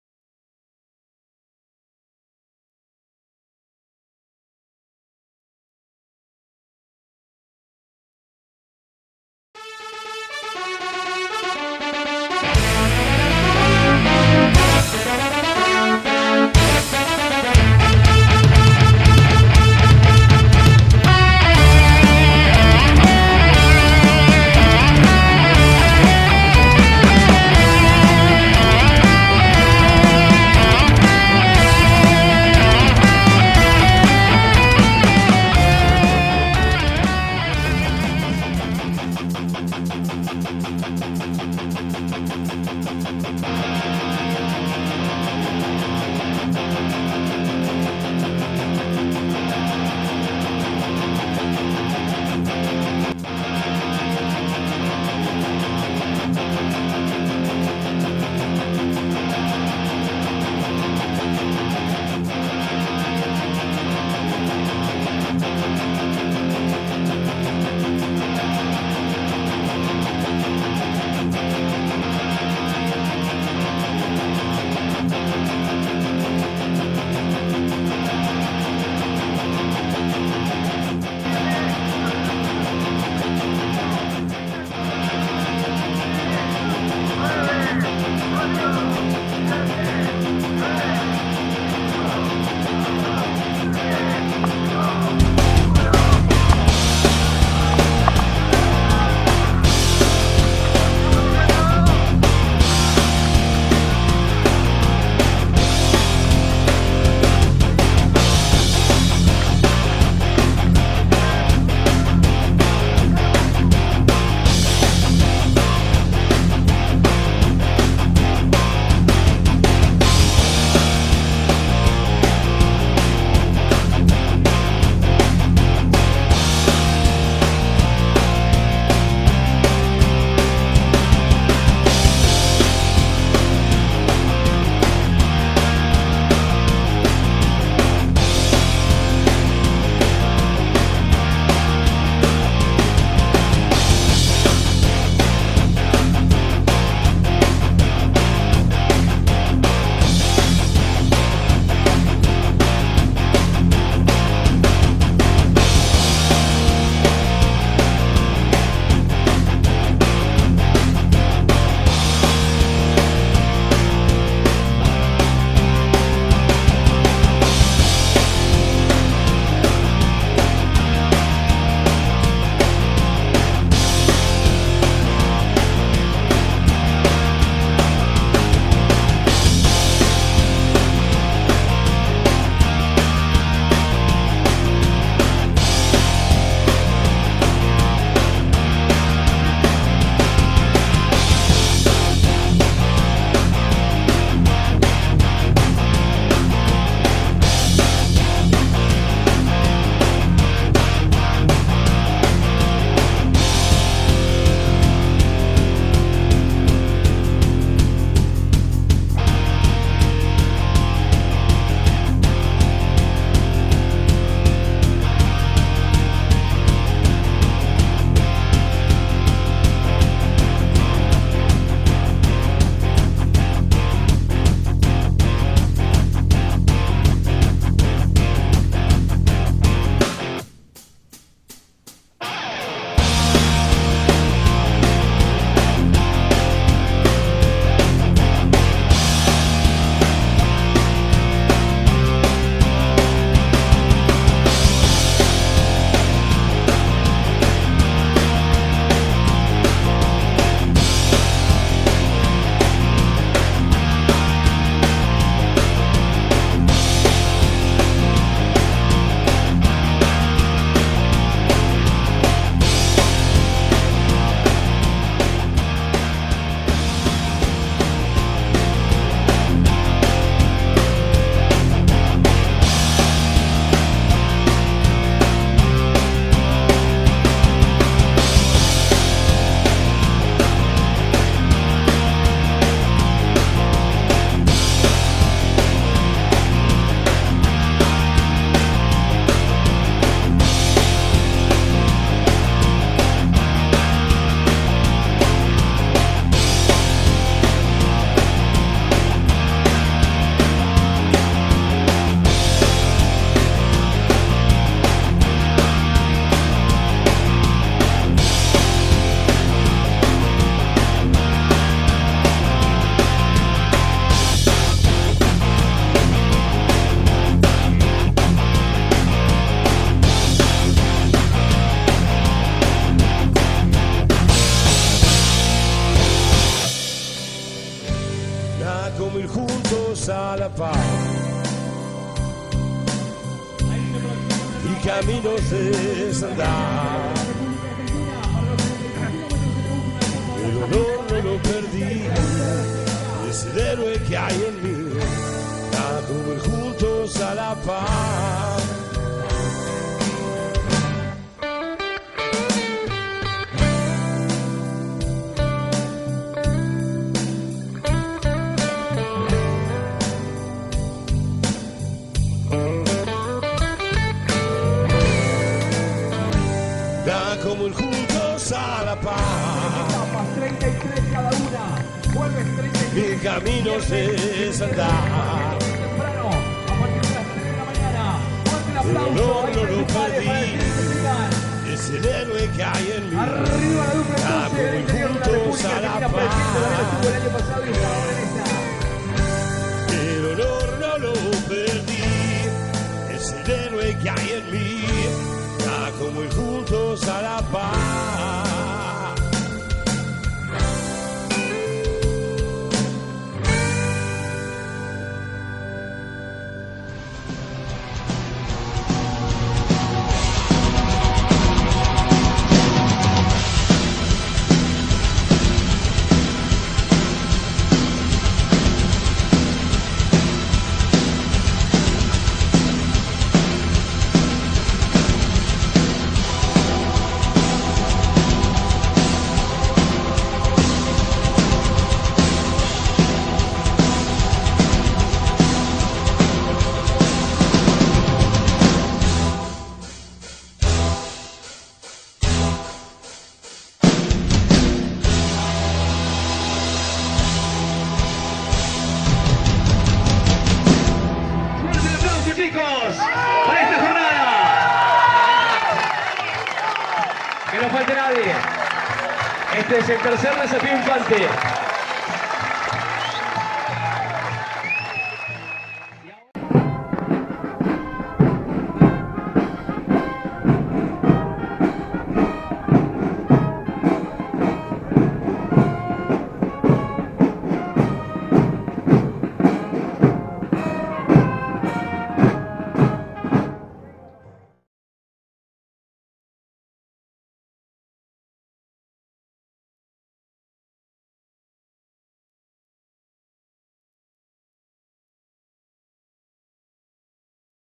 DESAFIO INFANTE 2016 TERCERA ETAPA
8, 9 y 10 de Diciembre del 2016, Tercera Ultramaraton 100 K
" Desafio Infante ", organizada por el Regimiento de Infanteria Mecanizado 6 de Toay, La Pampa .
Este es un resumen de la tercera y ultima etapa de 33 K.